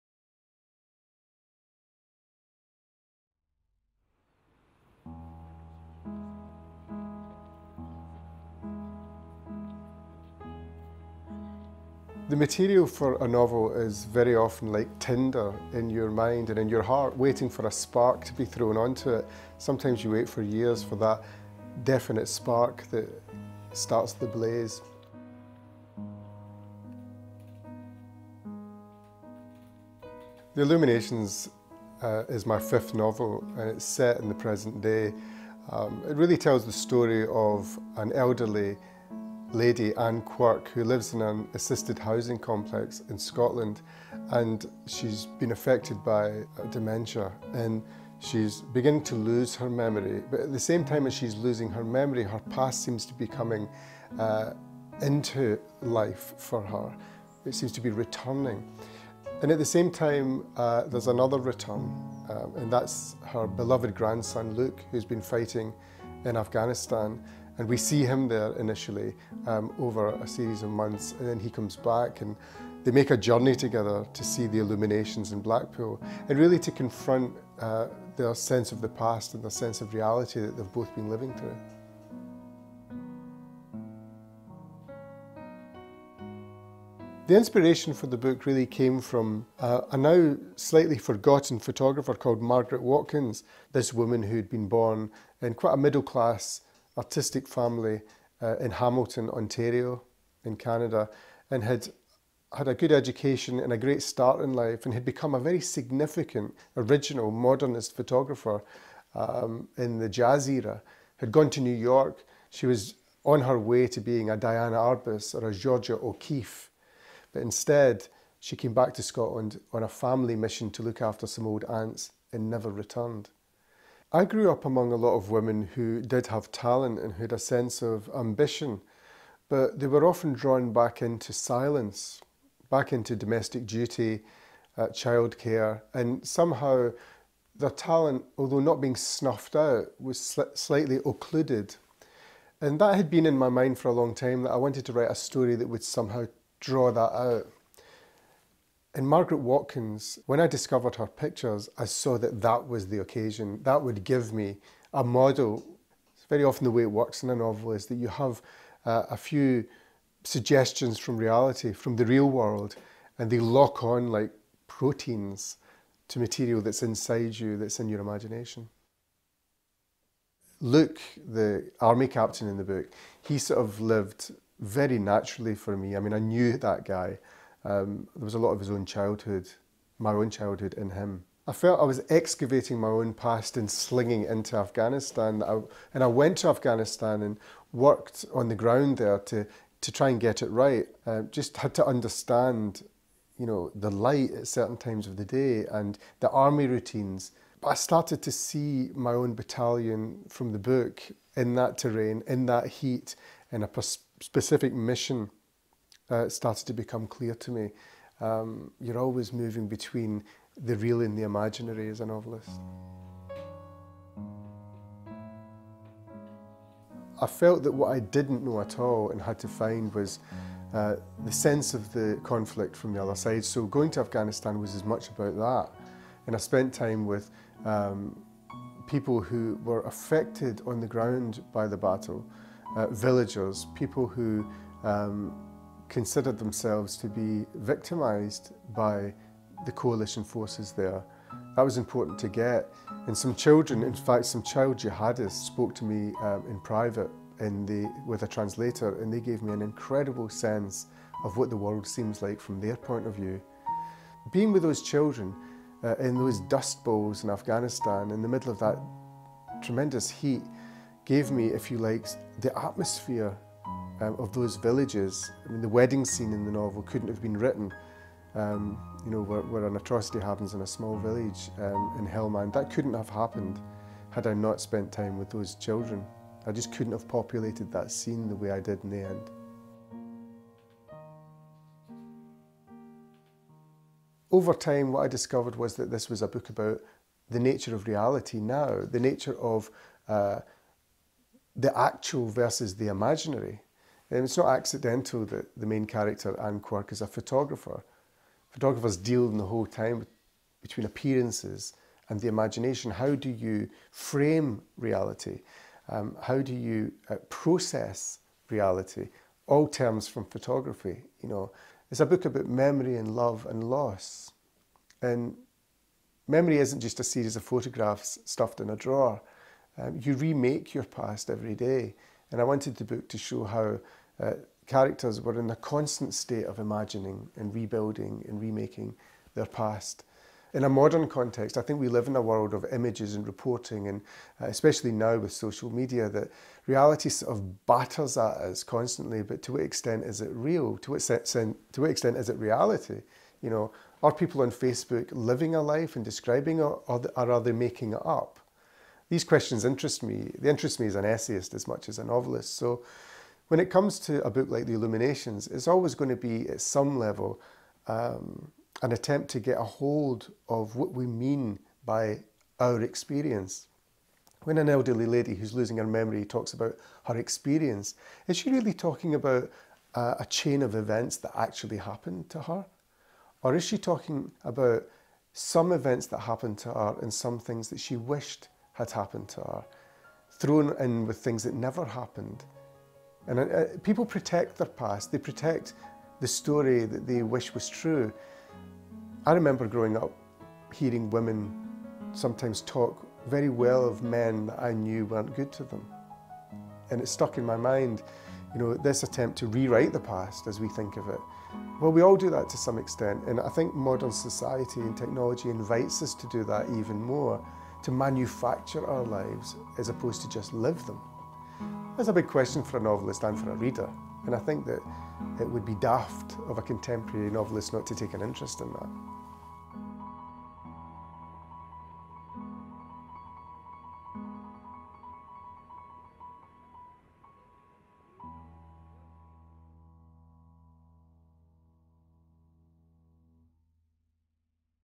Andrew O'Hagan on 'The Illuminations' | FT Life
Author Andrew O'Hagan explains how an early 20th century female documentary photographer and his time spent with child jihadis in Afghanistan influenced the lead characters in his latest book ‘The Illuminations’.